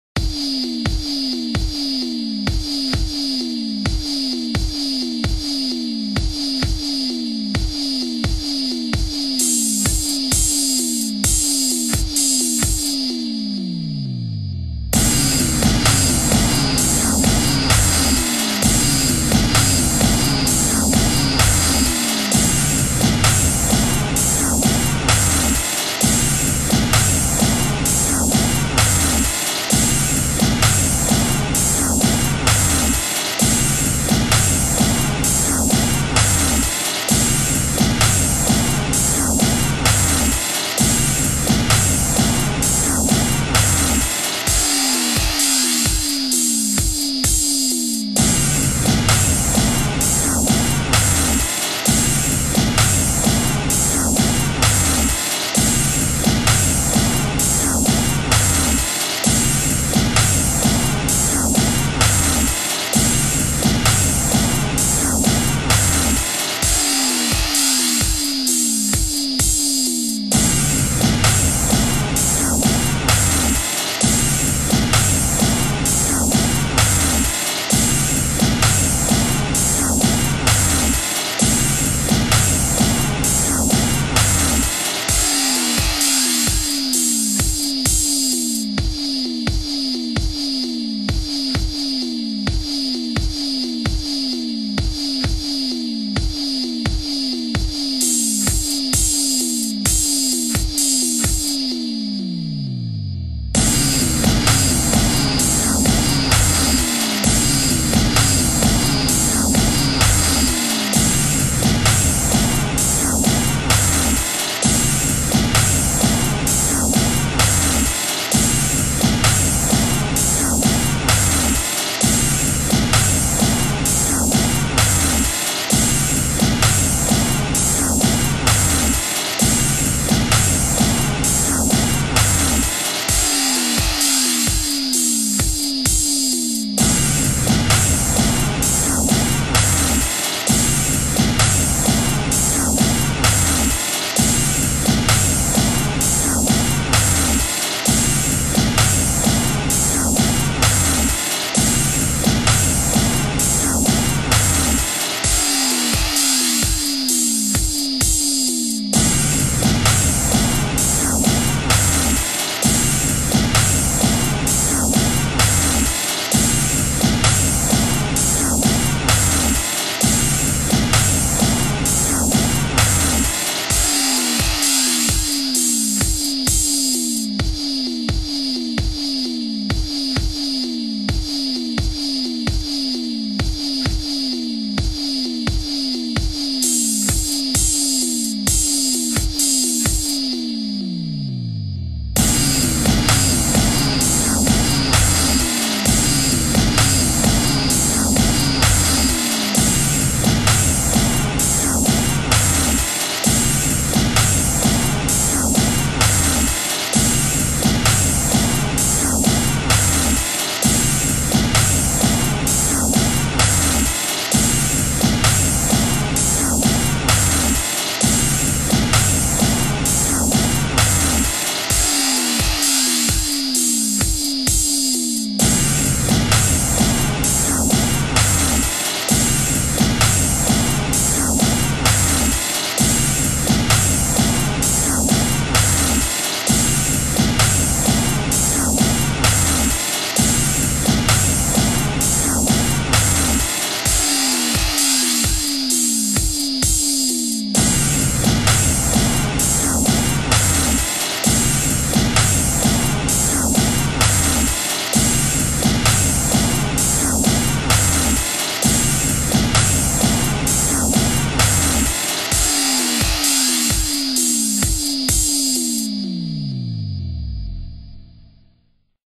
Trippy Aztec Wavy Style Rap Beat
Trippy Wavy Aztec style beat that sounds like a Triple C Dose.

future releases are beat tapes that will be 99 cents in a range of moods and styles, colors and flavors, like bubblegum, but only better.